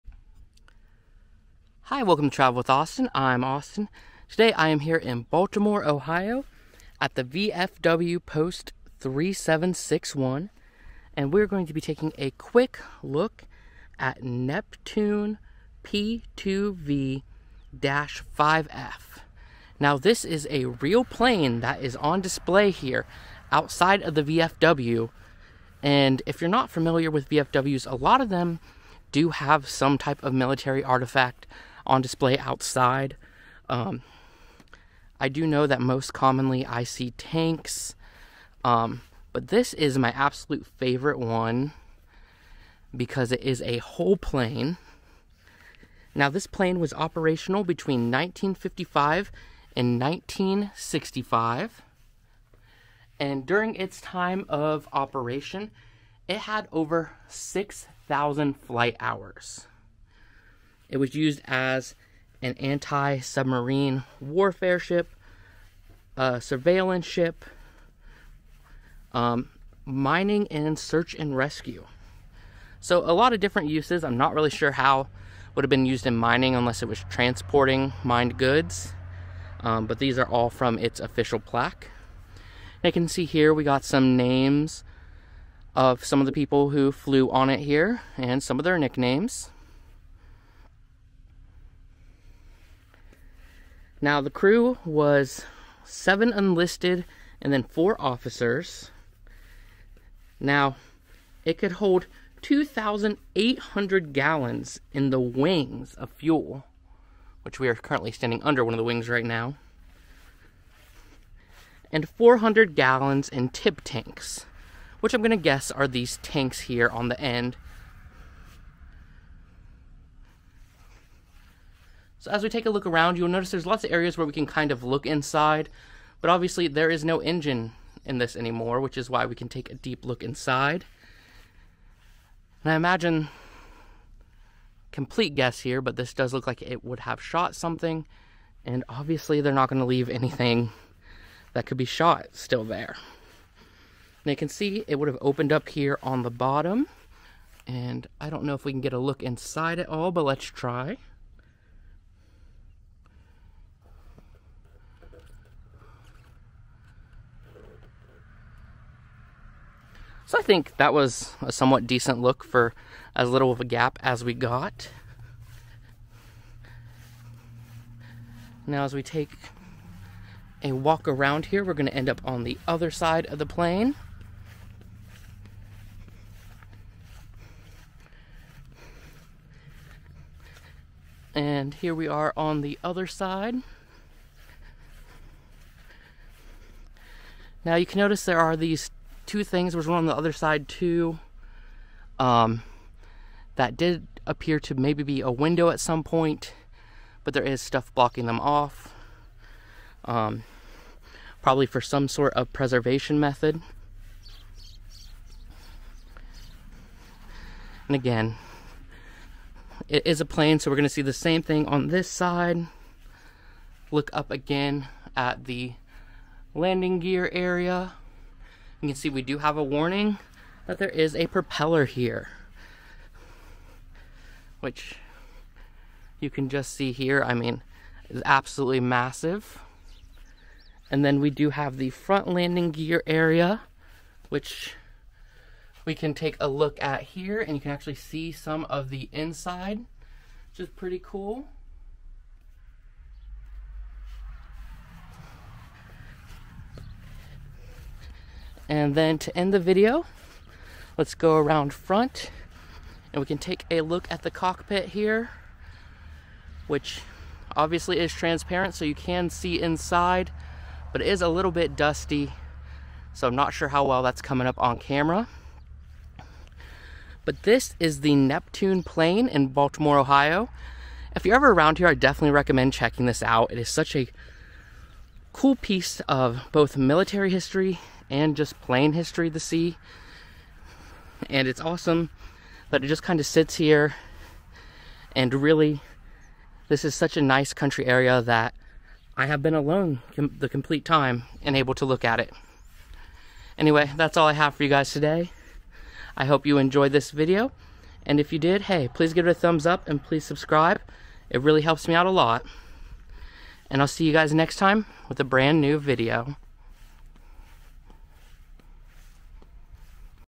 Neptune Military Plane
The Neptune Military Plane is a retired Lockheed P2V-5 Neptune maritime patrol aircraft that is now on display at the VFW post in Baltimore, Ohio. In this video, we take a closer look at this impressive aircraft and learn about its history.